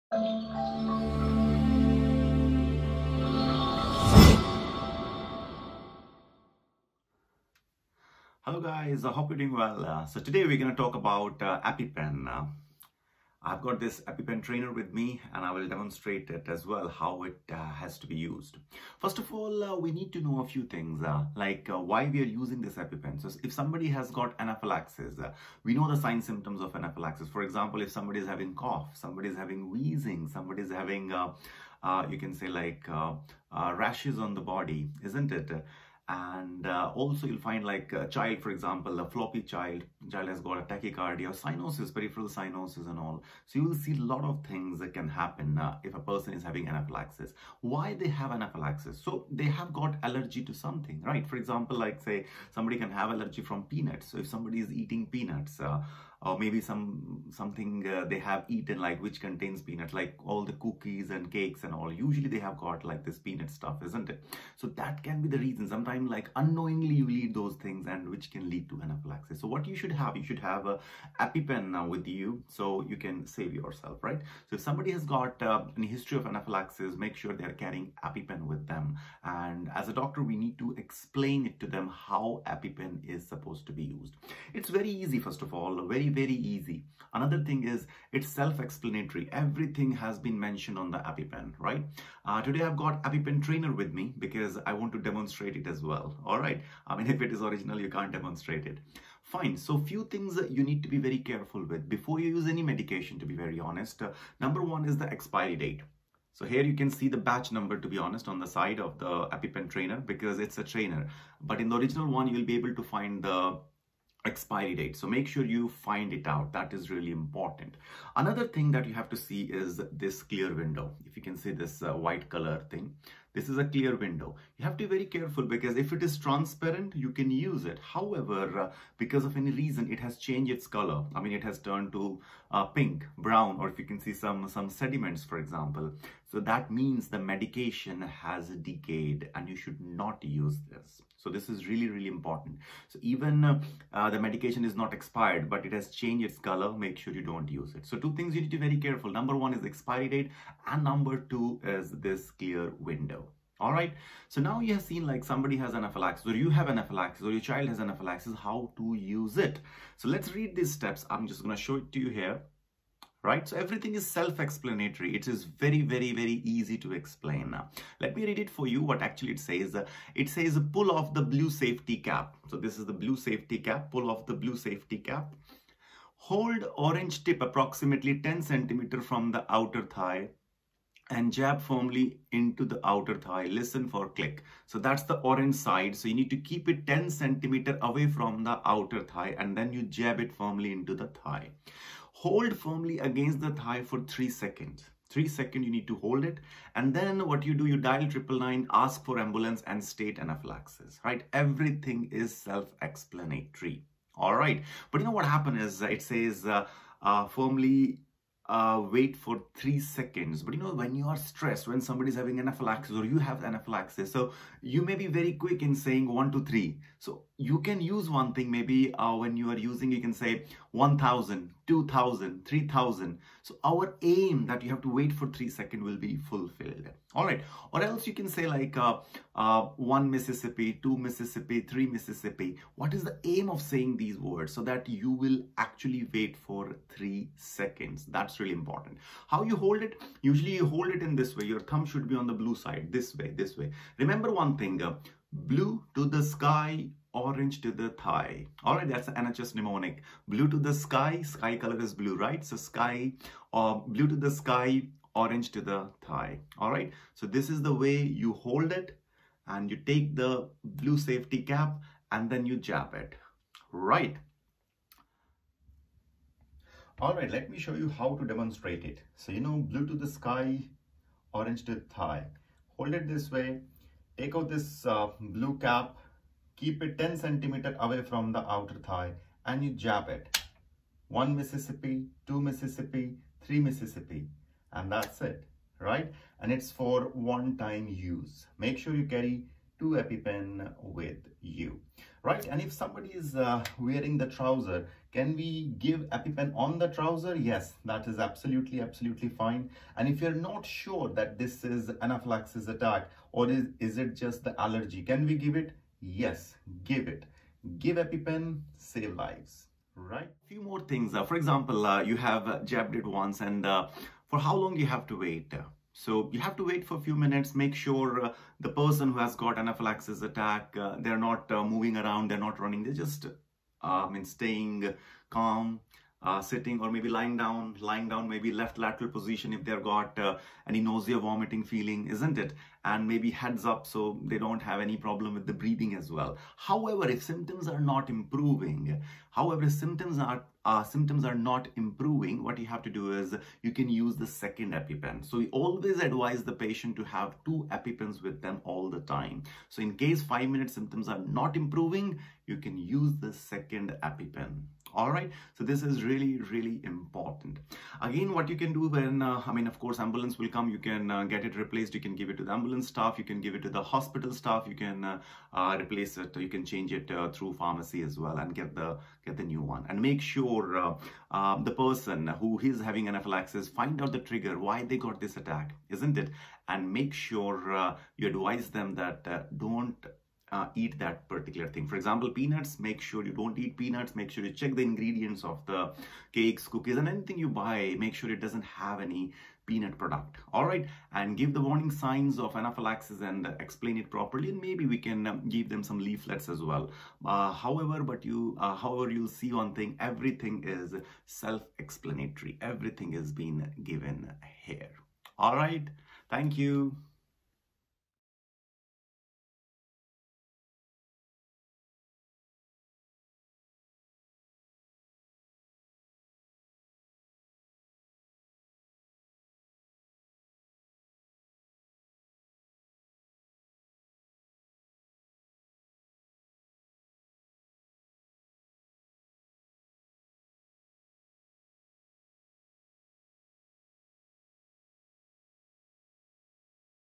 How to use an EpiPen by Dr. Ankur Garg | Aspire Education | PLAB2
In this Video, Dr. Ankur Garg explains the EpiPen its usage & best practices.